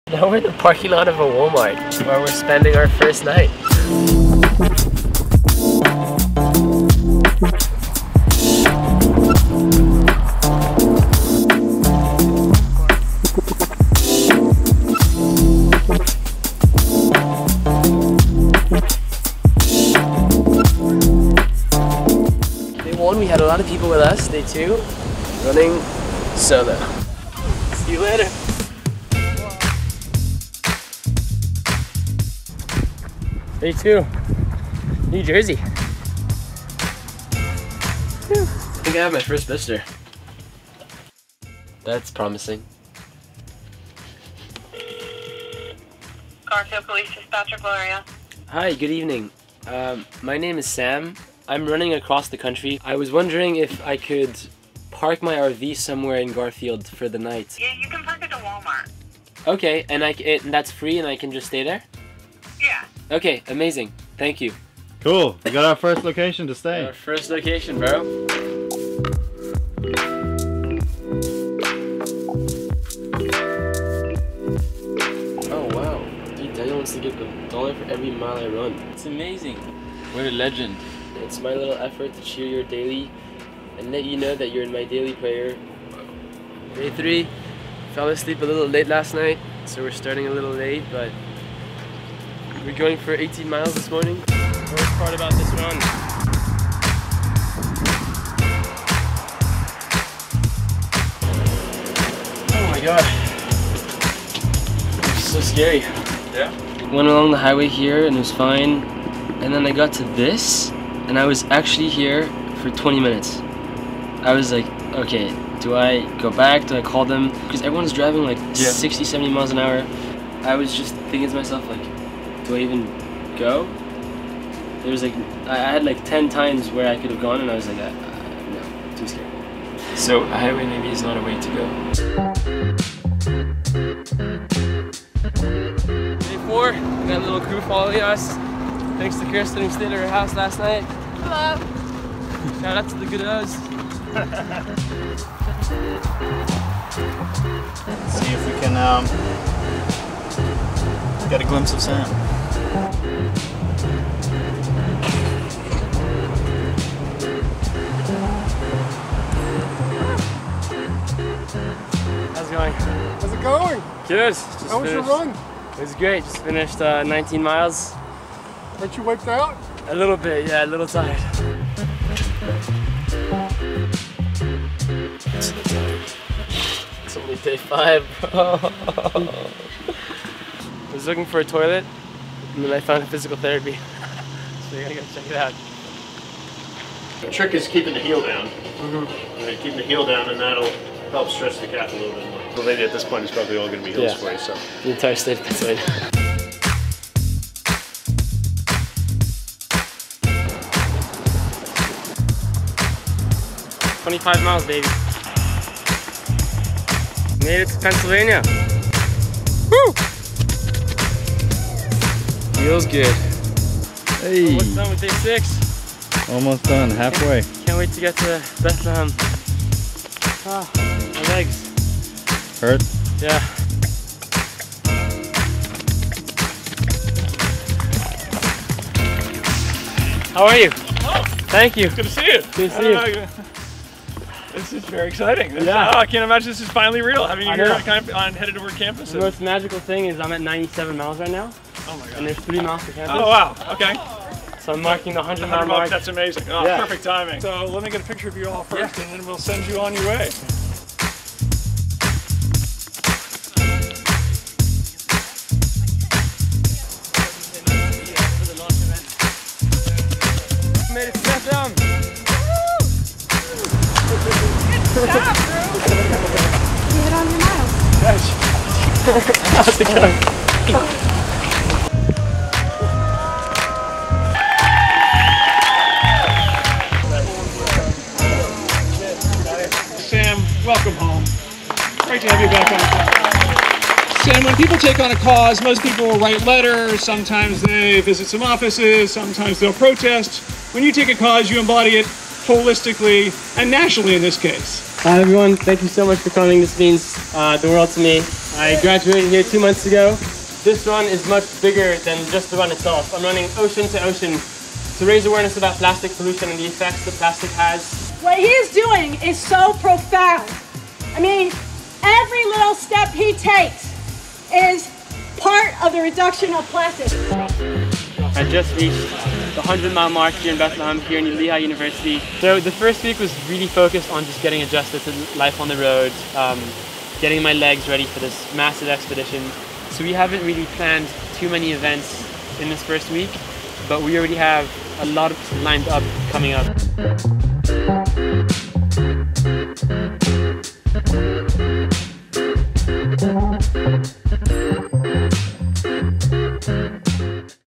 Running Across America Ep. 2 - 100 Miles 🏃‍♂️
First week down of his 3,055-mile journey across America on foot. Our co-founder Sam celebrates his first 100 miles closer to the Pacific Ocean! Last week, Sam began his epic journey from New York City. In this episode, Sam goes through his first week before stopping at his alma-matter, Lehigh University, where he hosts his first community talk.

A project by Make A Change, in collaboration with Parley For The Oceans and with the support of Adidas Running and StepChain.